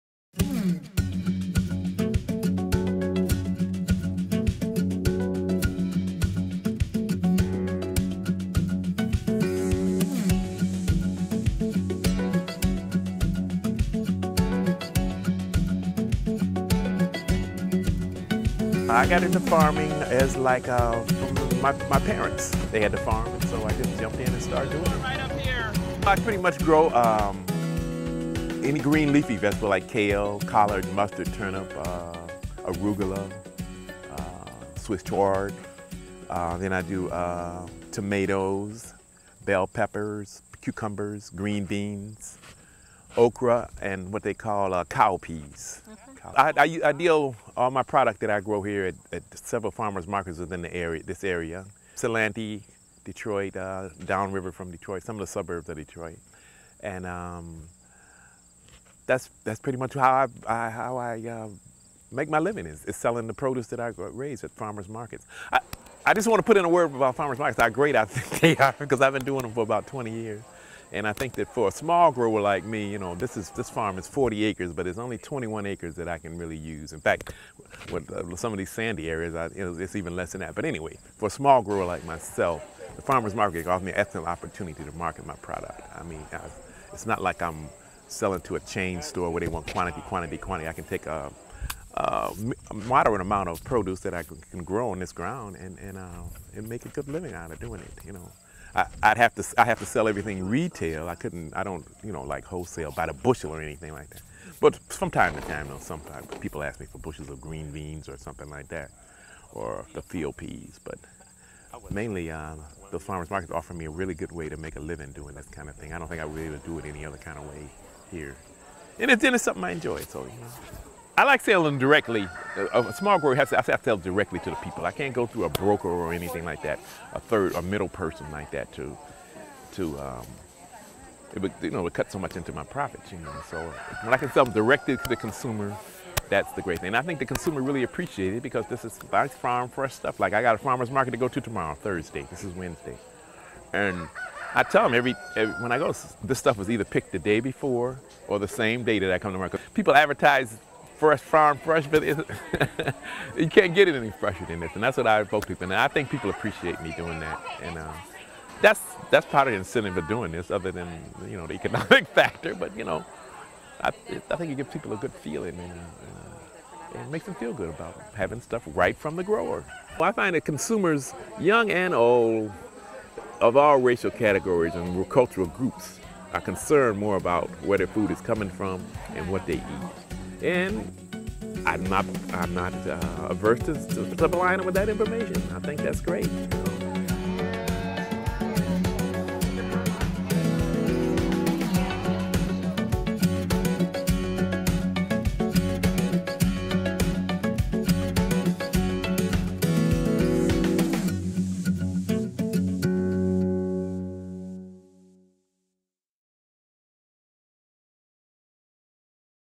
Michigan Good Food Charter Goal #2: Good Medicine Farm, Milan, Michigan
Good Medicine Farm is one example of a farm that is supporting Goal 2 of the Michigan Good Food Charter. Goal 2 states that Michigan farmers will profitably supply 20% of all Michigan institutional, retailer and consumer food purchases and be able to pay fair wages to their workers.

This video is the second in the series, Advancing the Goals of the Michigan Good Food Charter.